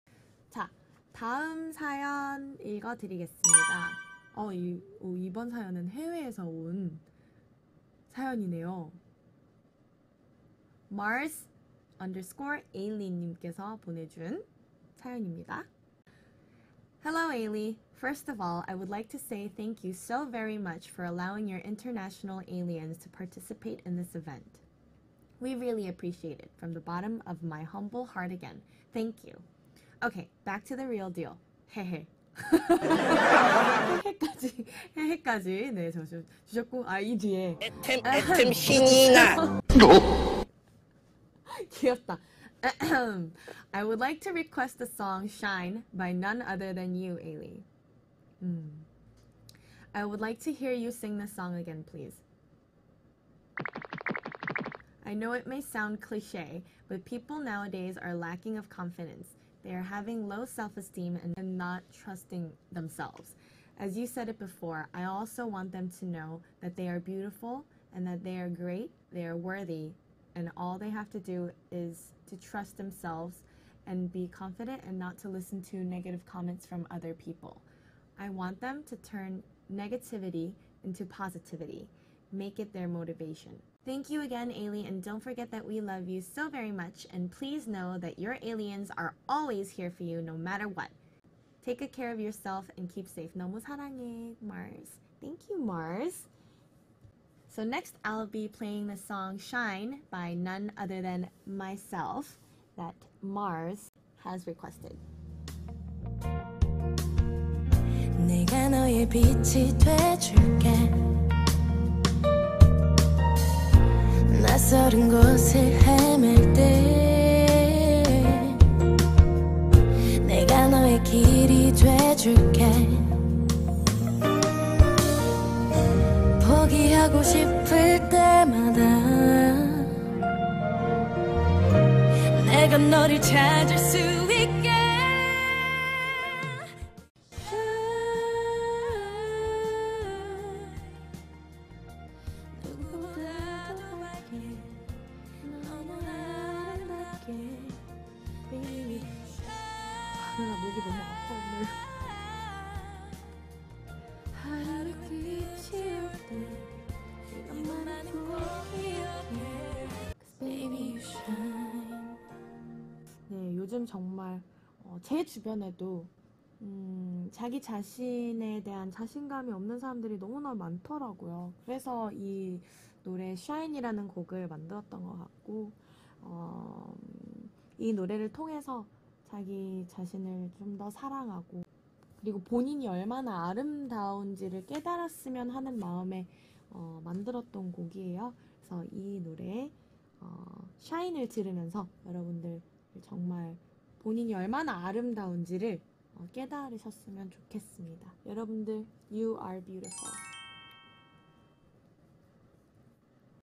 [AILEE]ilee night 일리 있는 밤 II
에일리의 일리 있는 밤!
이번엔 해외에서 슝 날아온 사연으로 시작됩니다!
“여러분은 아름답습니다!”
'오늘의 DJ 일리 있는 밤' 에일리입니다.

*이번 콘텐츠의 특성상 한글자막과 영어자막이 함께 게시되며,
유튜브 자막 시스템은 월요일에 업로드 됩니다🙏

This time, it starts with a story from abroad!
“You’re Beautiful”
Tonight's DJ ilee night AILEE

*The content is mixed with Korean subtitles and English subtitles. The subtitle system will be uploaded on Monday🙏